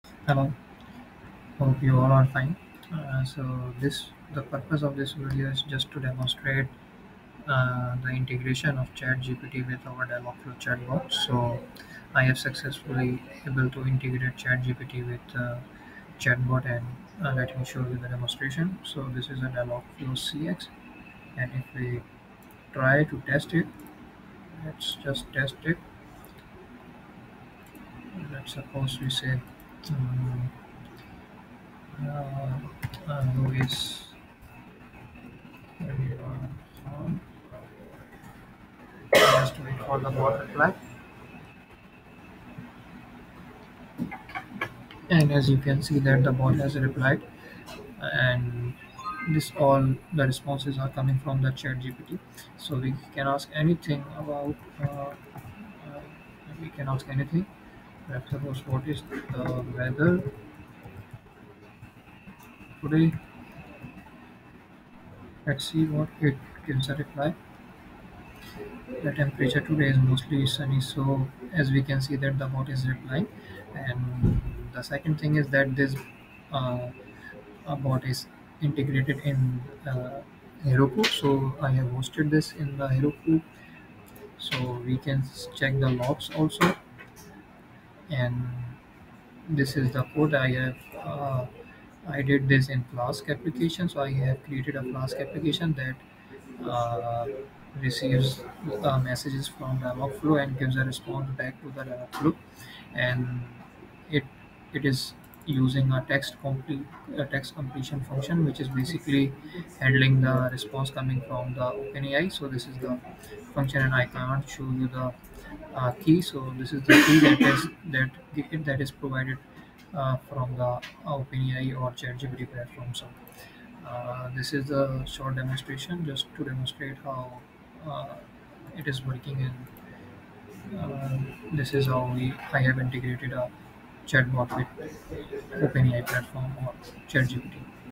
ChatGPT + Dialogflow CX + Heroku
This is a demonstration video about Chatgpt integration with Dialogflow CX and hosted in Heroku.